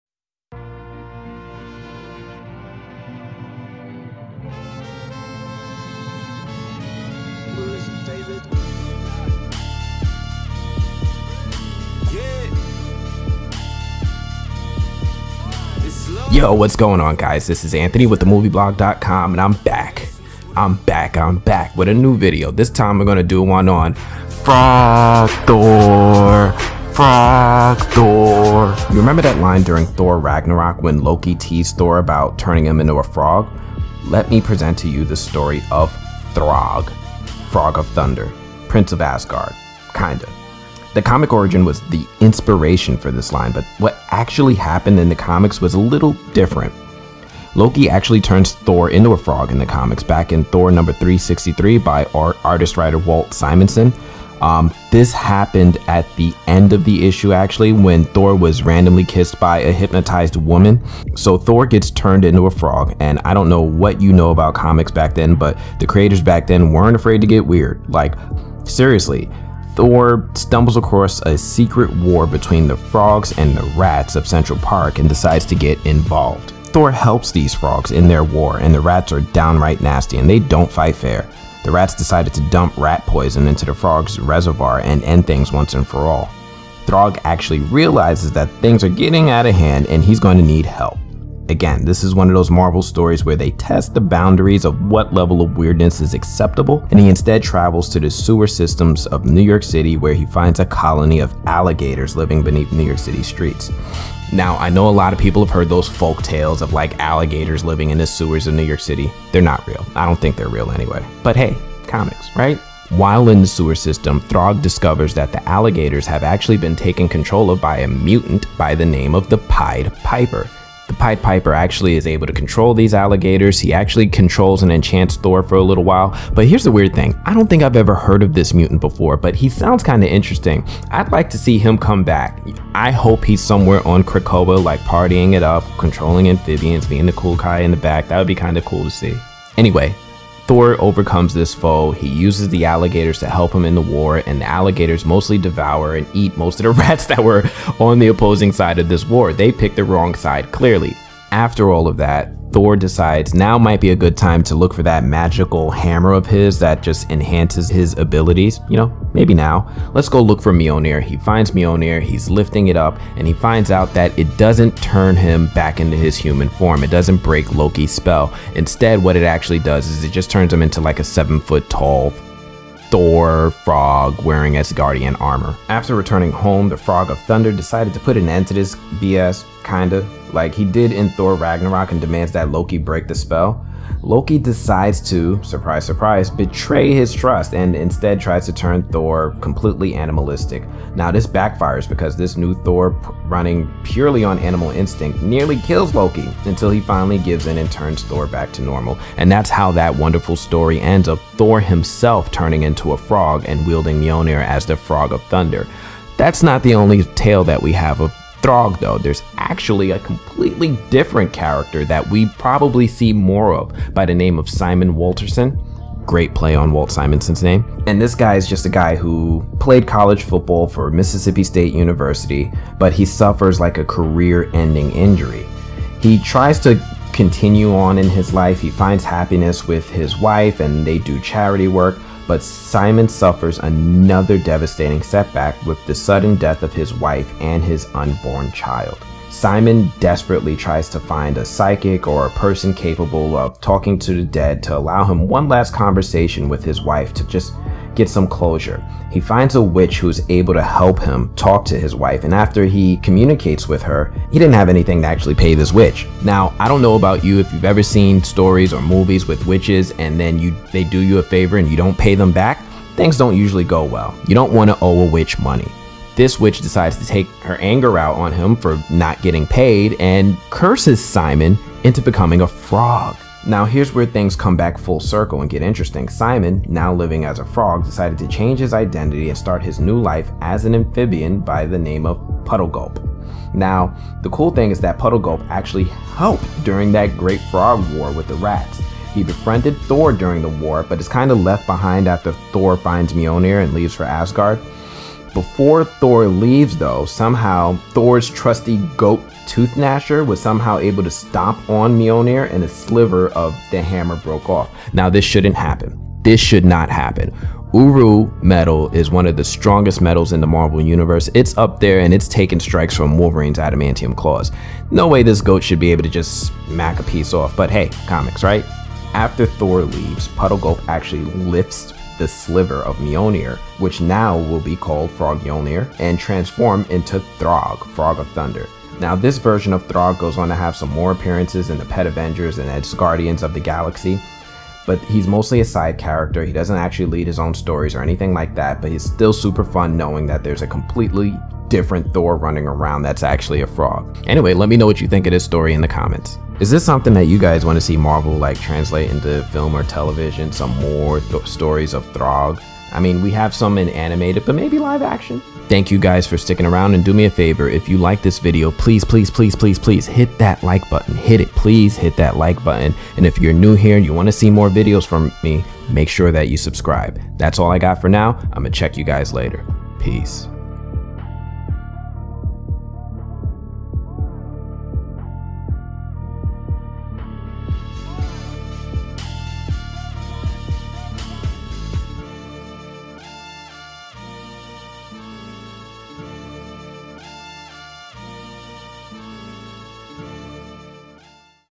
Thor Variant Throgg Frog of Thunder
The Movie Blog: Frog Thor made waves with that cameo in the Loki TV series. Now we're taking some time to review the comic book inspiration for that cute cameo and learn about Throgg Frog of Thunder